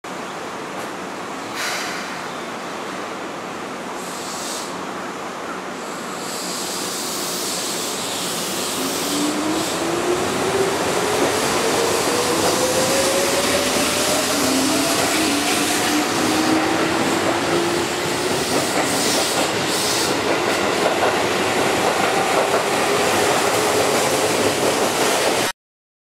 いわゆるハズレ快速急行 リニューアルしてもモーターは33年目のままの見かけ倒しの6011編成君の「快速急行 淀屋橋」
こちらは本日（5/29）の夕方からの洛楽の第4弾とセットのD1701Aの便になります(爆）